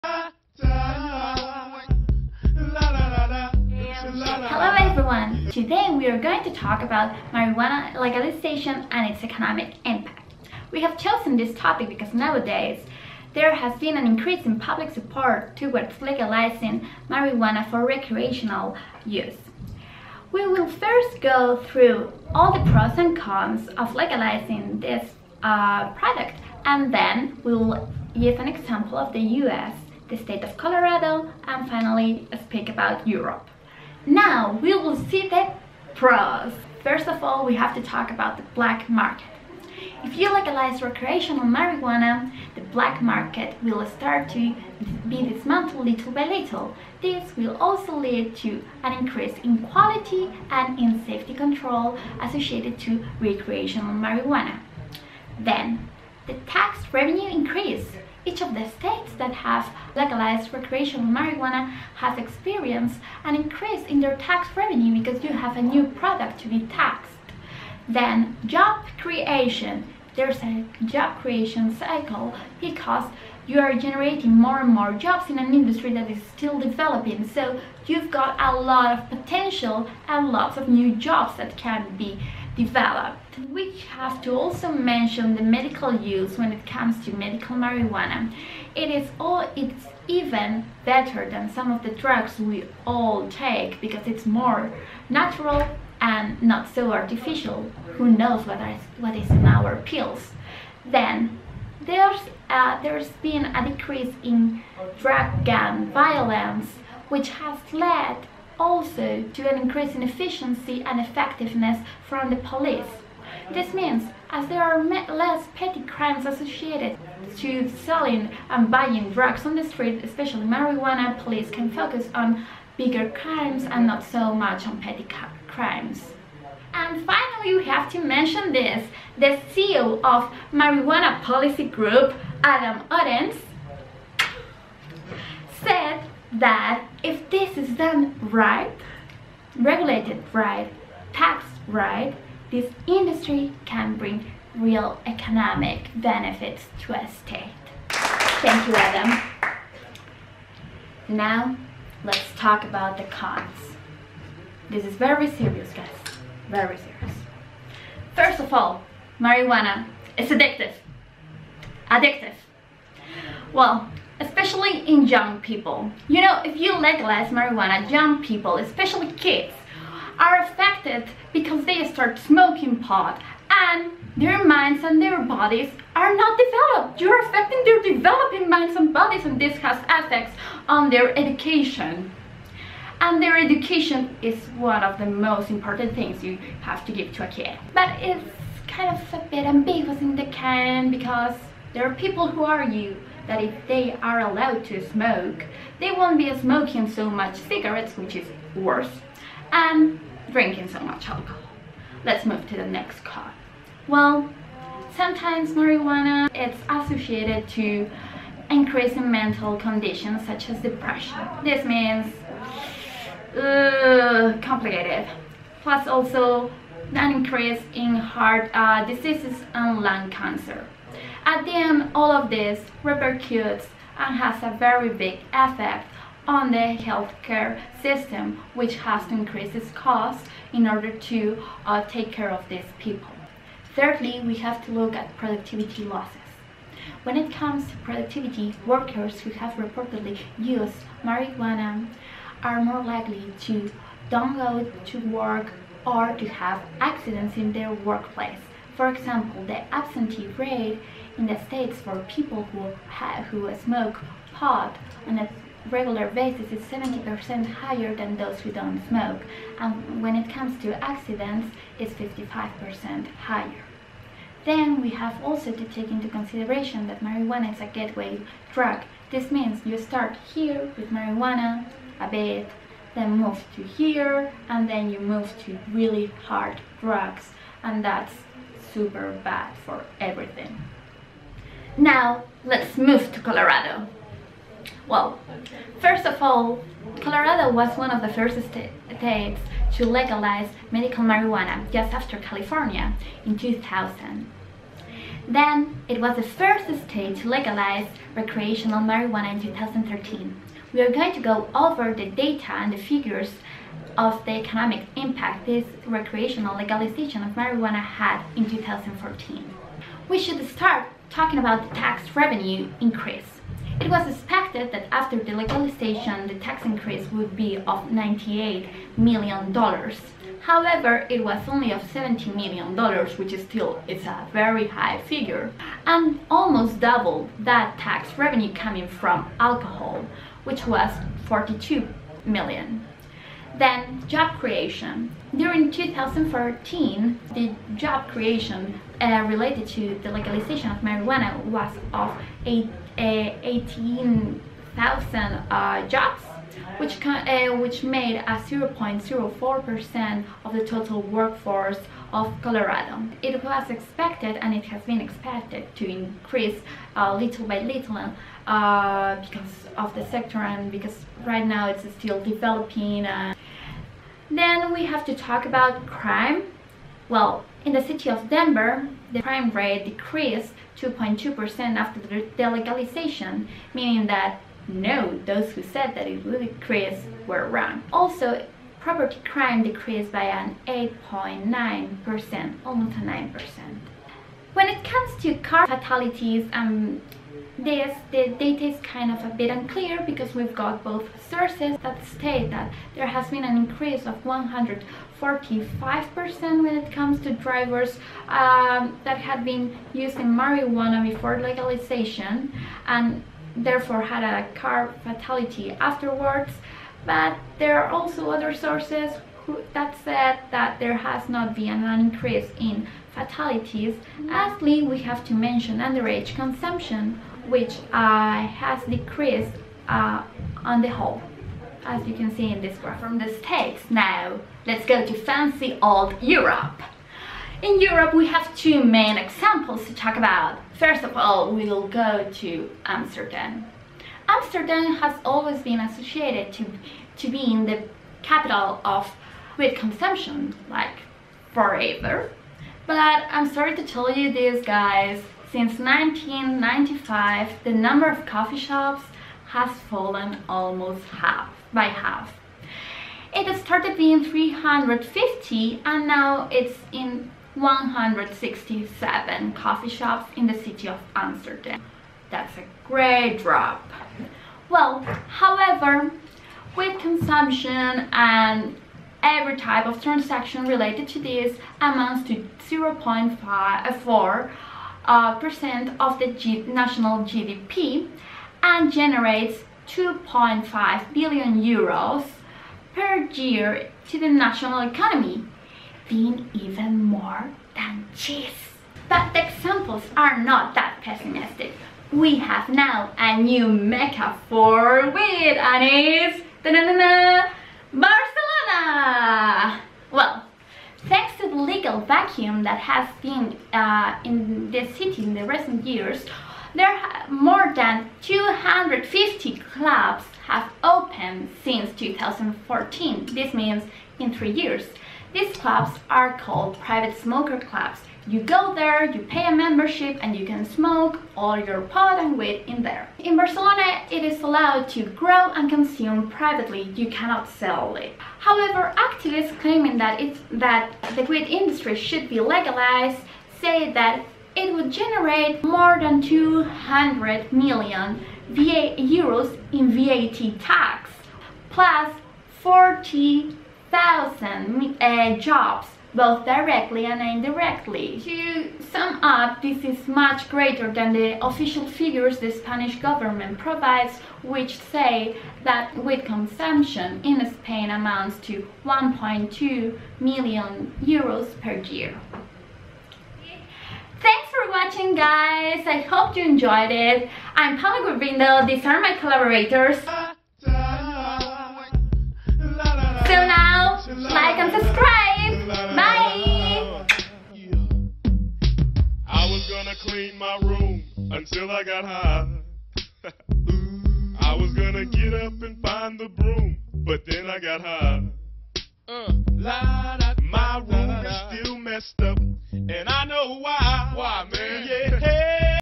Marijuana Legalization and its Economic Impact
Paula Gurbindo, Gonzalo Suarez, María Vargas, Pilar Zhou.

BIBLIOGRAPHY.

Bas, Tanya, “ Colorado Raised more Tax Revenue from Marijuana than from Alcohol”, Time, 18.MAt 2016.

Evans, David. G. The Economic Impacts of Marijuana Legalization,  The Journal of Global Drug Policy and Practice. 30. Dec. 2013.

Francos, Anthony. “12 Pors and Cons of Marijuana Legalization”, Honest Marijuana Company, 11. Sep. 2017.

Heuberger, Brian. “Despite claims, data show legalized marijuana has not increased crime rates”, Colorado Politics, 22. March 2017.

Ingraham, Christopher. “ The Marijuana Industry created over 18,000 new jobs in Colorado last year”, The Washington Post, 27. Oct.

Krishna, Mrinalini. “The Economic Benefits of Legalizing Weed”, Investopedia. 20 Apr. 2017. Retrieved from : •
Mattock, Carol. “Barcelona is Fighting an Overdose of Cannabis Clubs”, Bloomberg Businessweek, 13. June 2014.

Migoya, David. “Exclusive: Traffic fatalities linked to marijuana are up sharply in Colorado. Is legalization to blame?” , The Denver Post, 27. Aug. 2017.

Sully, Jacob, “As Colorado Loosened its Marijuana Laws, Underage Consumption and Traffic Fatalities Fell”, Forbes, 11. Aug. 2014.

Various, “It’s official: drugs, prostitution boost Durch economy”, Reuters, 25. June 2014.

Various, The Economic Impact of Marijuana Legalization in Colorado, The Marijuana Policy Group. Oct. 2016.